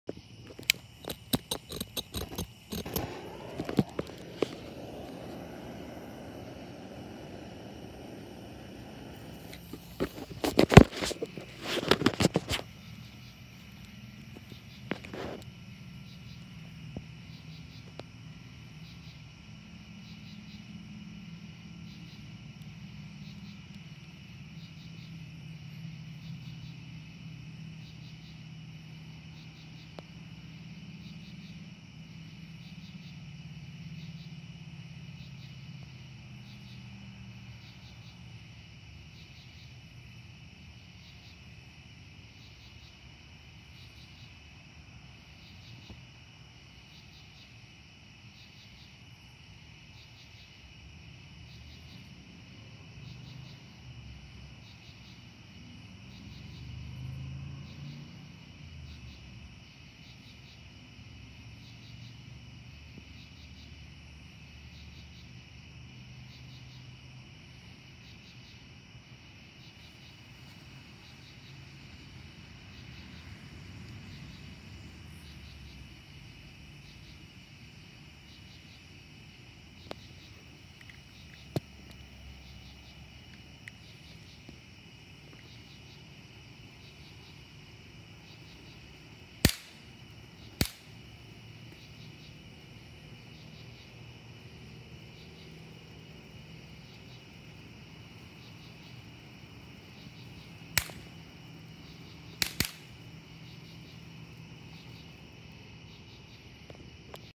Homemade three stage lance firework.
Marine flare and commercial fireworks disassembled to give the three colors.  Yes, the second stage is green, the camera can’t see it for some reason or another.